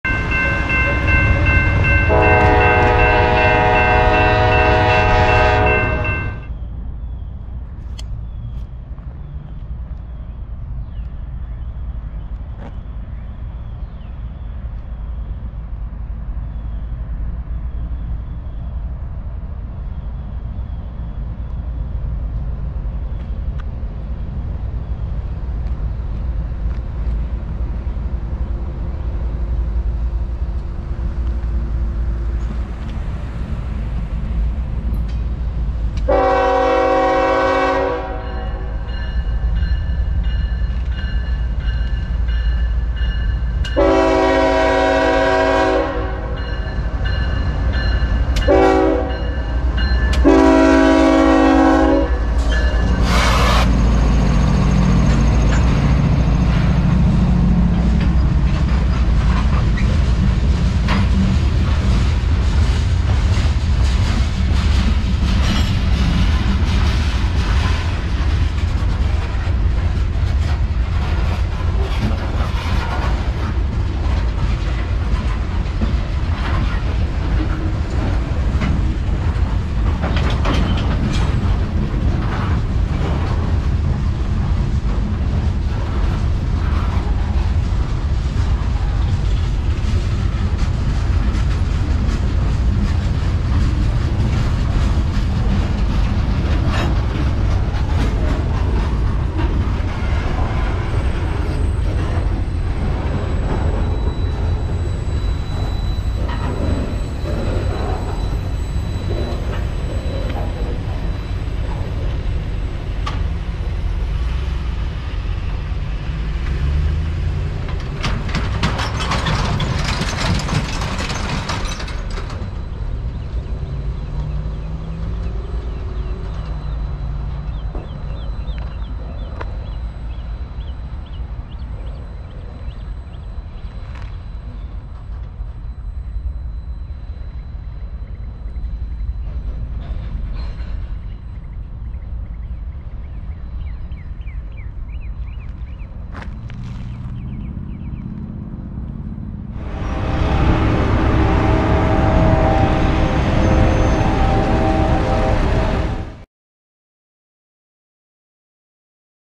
S01E119 John Easley IS Claw Hammer (Norfolk Southern, Manifest, ES44AC, Graffiti)
Another minute and I would have missed this delightful NS P75 local. Not too short, nice mix of cars, and some cool tagging. Please click the thumbnail, watch, and enjoy. Subscriptions are welcome. Thanks!